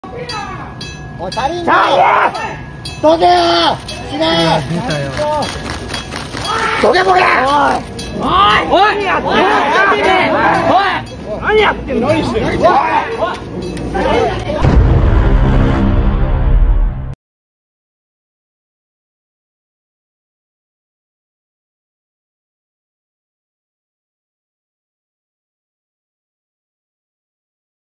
coming soon!?海外の暴走族🇩🇰のちょい見せ…Bosozoku is Gentleman...🇯🇵
海外の暴走族。デンマークで暴走NOWです。日本伝統文化広めるため、今夜も恥晒します。
Japanese culture!? Bosozoku is Gentleman...